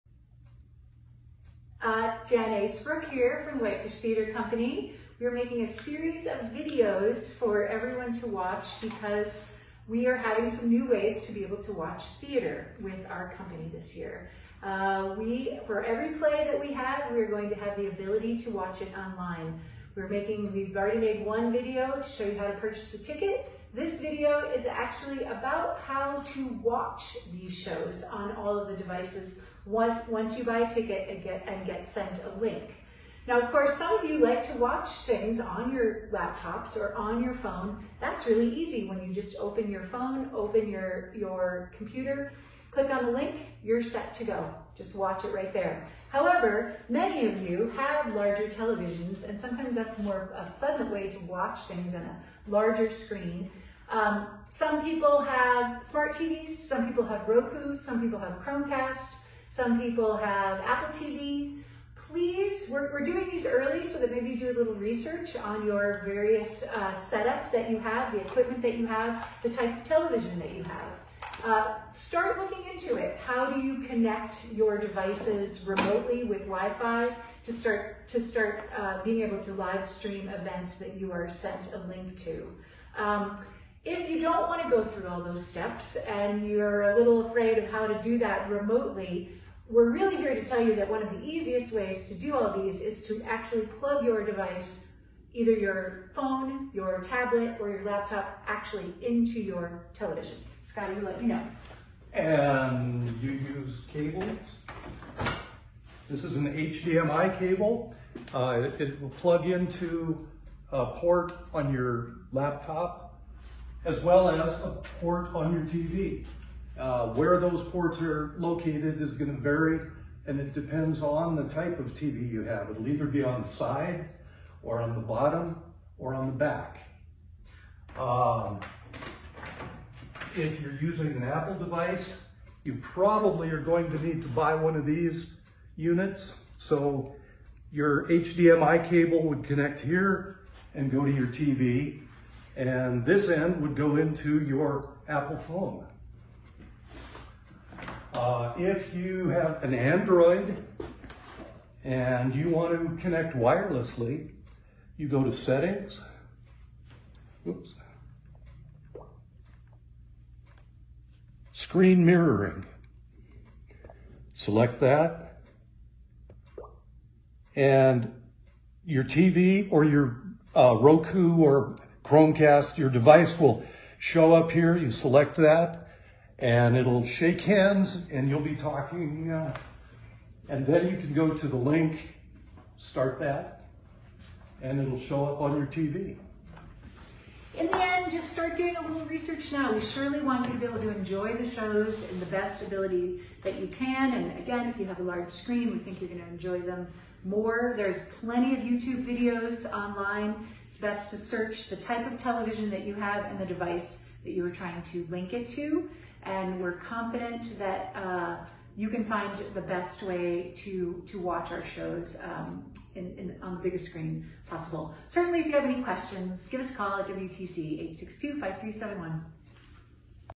How To Watch Play On Your TV
Brief instructions on how to watch a WTC play (after purchasing a ticket for an online performance) on your television or other device.